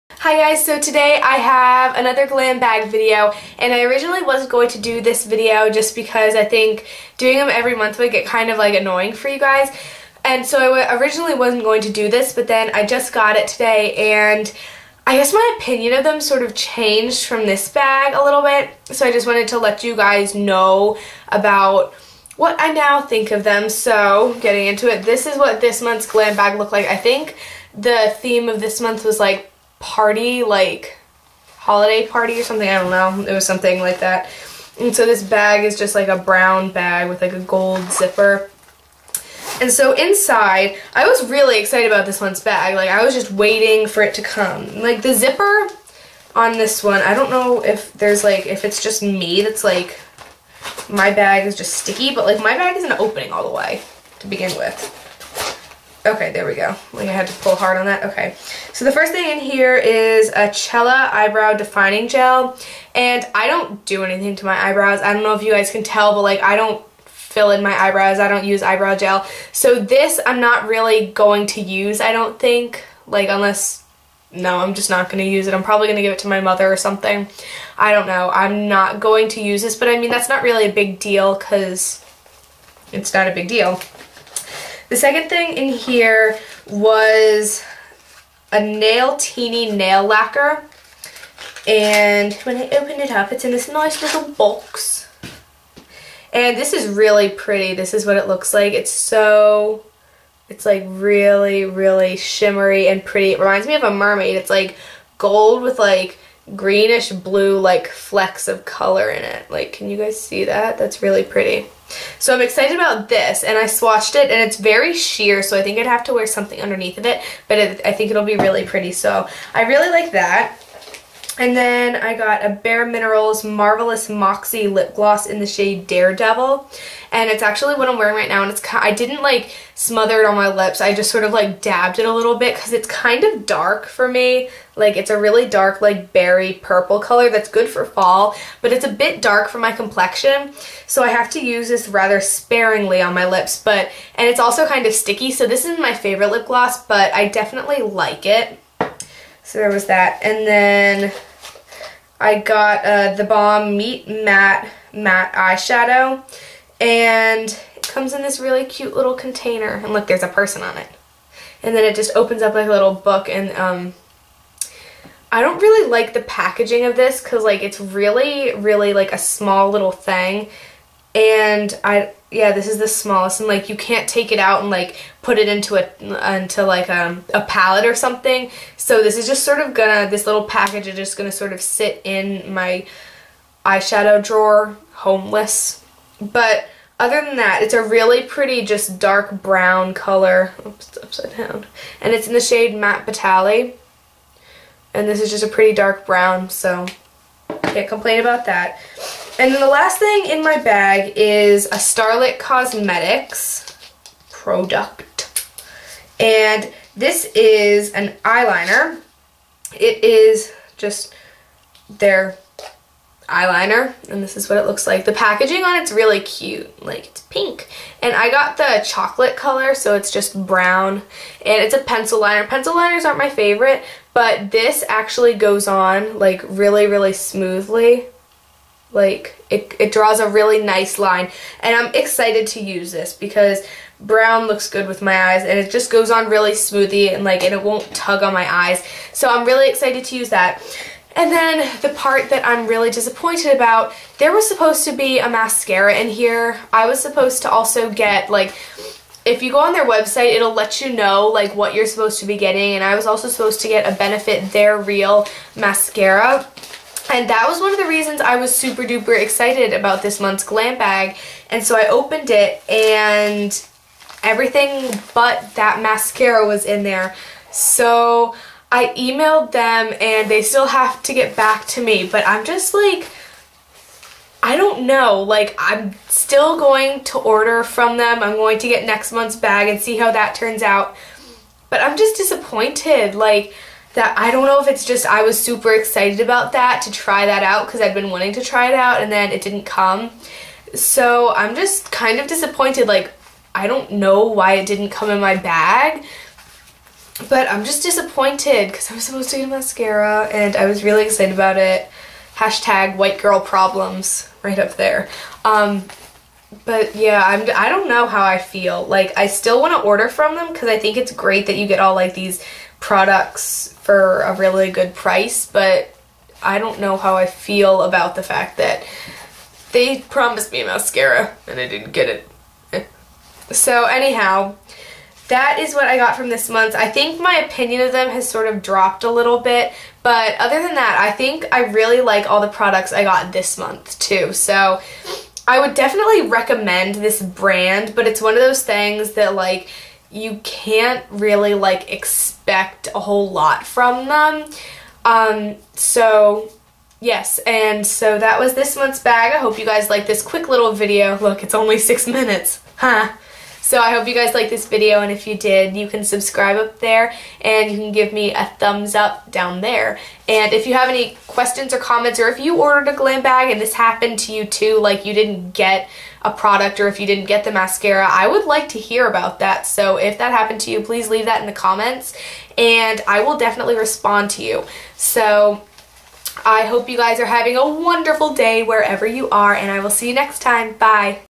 Glambag November 2012
So it turns out you either got the eyebrow gel or the mascara. I'm still peeved because that wasn't made apparent, but oh well...